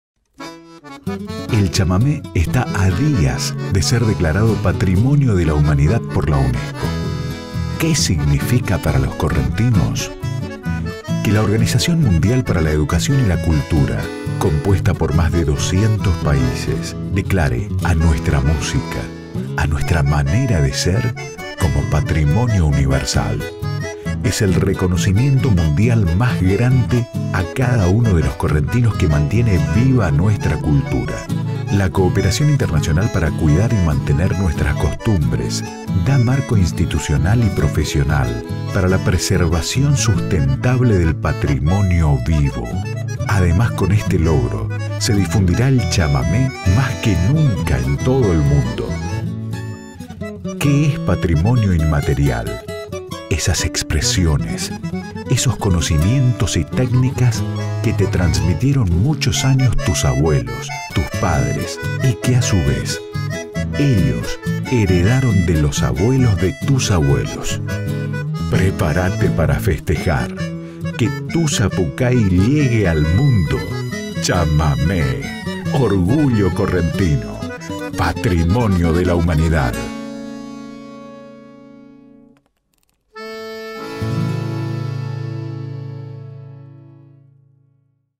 Porqué es importante que la declaración del Chamamé como Patrimonio Inmaterial de la Humanidad
Video gentileza Gobierno de Corrientes